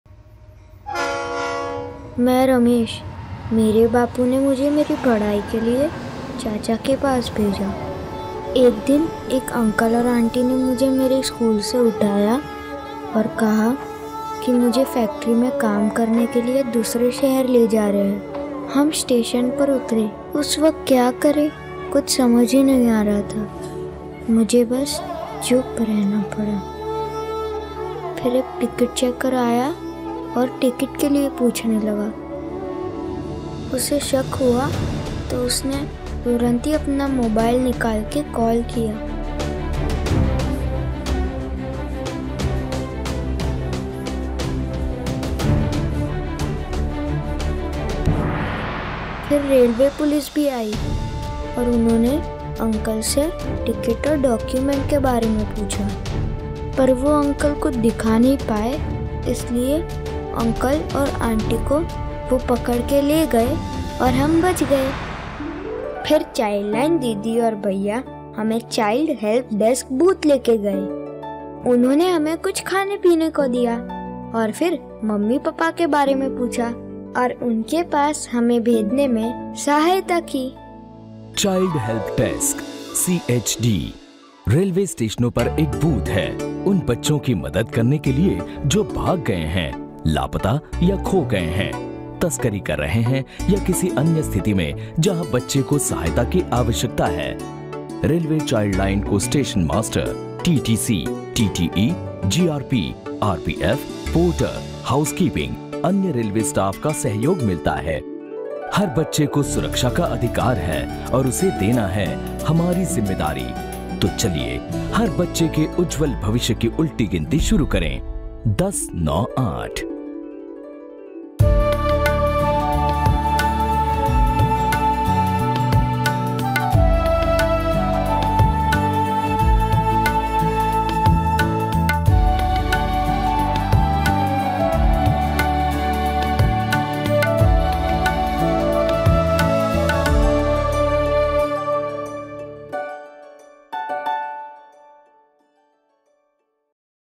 Child Help Desk – Railway CHILDLINE || An initiative by CHILDLINE India Foundation.
The Child Help Desk (CHD) is a booth that has been established at railway stations across India to provide assistance to children who are runaways, missing/lost, trafficked, or in any other vulnerable situation where they require support. A Standard Operating Procedure was formulated in March, 2015 and a Memorandum of Understanding signed between Ministry of Railways and Ministry of Women and Child Development on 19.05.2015. Towards implementation of the above SOP, one of the operative instructions was to set up Child Help desks/ Kiosks/ Booths across railway stations. These are manned by trained CHILDLINE personnel, which provide assistance to children in rehabilitating and restoring children. The CHD is operational 24x7 at 134+ railway stations. It is imperative to highlight the significance of the CHD and the CHILDLINE team, which operates in the busy routes of the Indian Railways to safeguard children. Being in a high footfall area as a railway platform, not only does this mechanism prevent abuse or link a child with statutory bodies and services, it also provides large scale awareness about how a child can be helped and thereby promotes civic participation. The CHILD Help Desk at railway stations has assisted may children who were lost or missing and prevented child from being trafficking.